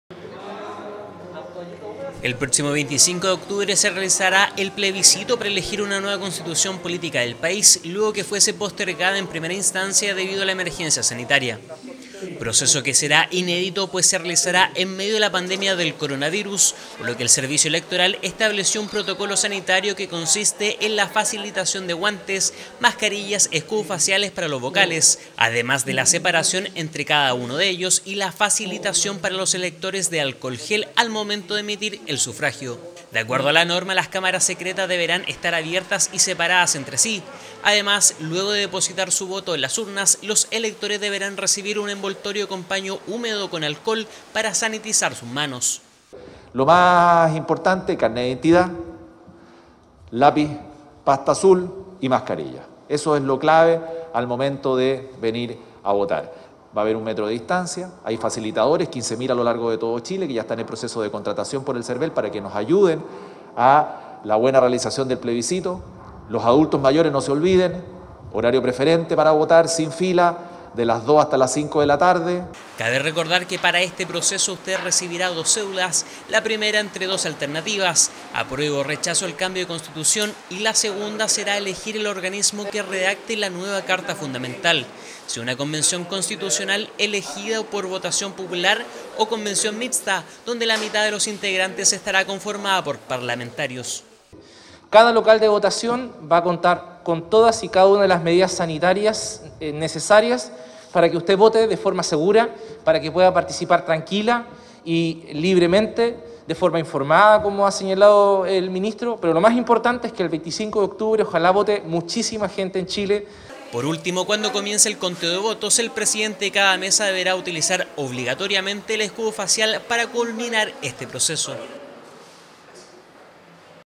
PROTOCOLO SANITARIO PARA UN PLEBISCITO SEGURO EL 25 DE OCTUBRE
El ministro secretario general de la Presidencia, Cristián Monckeberg, junto al intendente de la Región Valparaíso, Jorge Martínez, y el alcalde de esa ciudad, Jorge Sharp, visitaron durante la mañana un centro de votación para mostrar las medidas que se tomarán con el fin de tener un plebiscito seguro el 25 de octubre